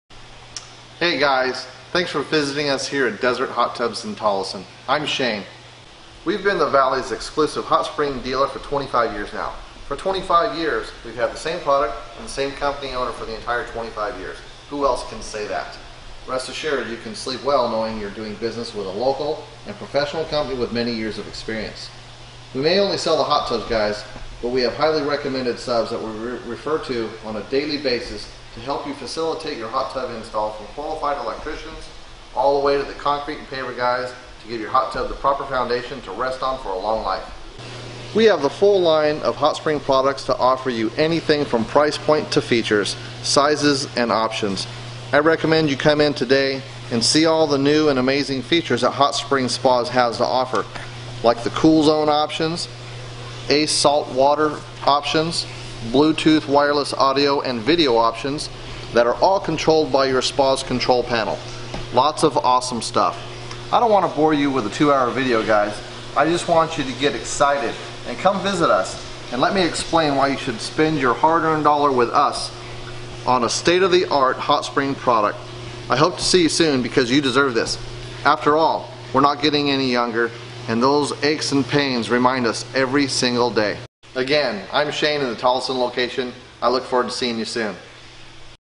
Desert Hot Tubs Tolleson AZ Hot Tub Showroom
Desert Hot Tubs Tolleson AZ - Meet Shayne, the manager of Desert Hot Tubs in Tolleson AZ. Stop in to see how the Worlds #1 Best Selling Hot Tub Brand can dramatically enhance your life. Desert Hot Tubs Tolleson AZ Showroom Tour.

Hey guys, thanks for visiting us here at Desert Hot Tubs in Tolleson. I'm Shayne.

We have been the Valley's exclusive Hot Spring Dealer for twenty five years now.

For twenty five years we've had the same product and the same company owner for the entire 25 years.

Who else can say that?

Rest assured you can sleep well doing business with a local and professional company with many years of experience!

We may only sell the Hot Tubs guys, but we have highly recommended subcontractors that we refer to on a daily basis to help you facilitate you hot tub install from qualified electricians all the way to the concrete and paving guys to give your hot tub the proper foundation for your hot tub to rest on for a long life.

We have a full line of HotSpring products to offer you anything from price point to features, sizes and options.

I recommend you come in today and see all the new and amazing features that HotSpring Spas has to offer.

Like the cool zone options, salt water options, blue tooth audio and video options that are all controlled by your spa's control panel. Lots of awesome stuff!

I don't want to bore you with a two hour video guys, I just want you to get excited and come visit us and let me explain why you should spend your hard earned dollar with us on a state of the art Hot Spring product I hope to see you soon because you deserve this!

After all we are not getting any younger and those aches and pains remind us every single day.

Again, I'm Shayne at the Tolleson location, I look forward to seeing you soon.